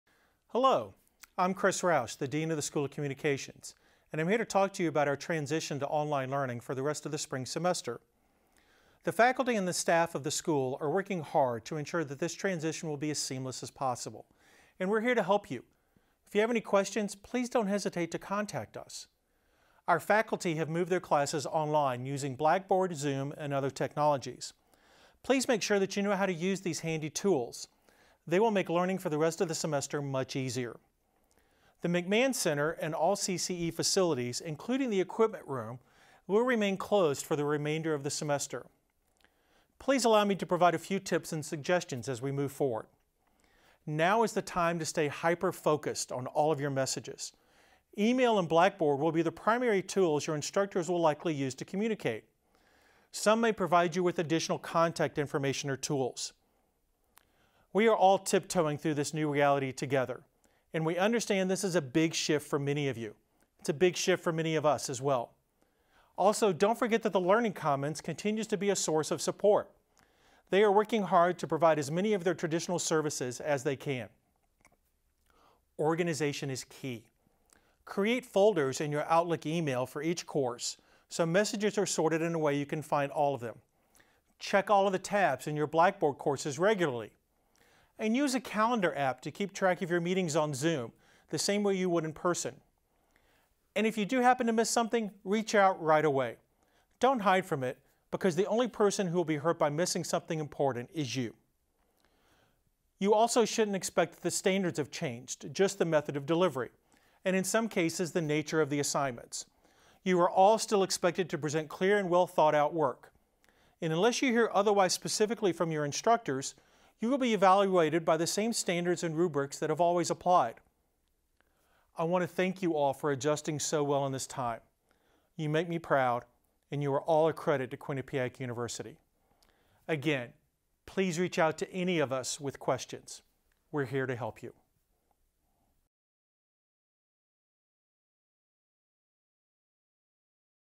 Dean Chris Roush SOC Announcement
Chris Roush, Dean of the School of Communications, addresses the students of the school to help explain the new online learning environment that we now face.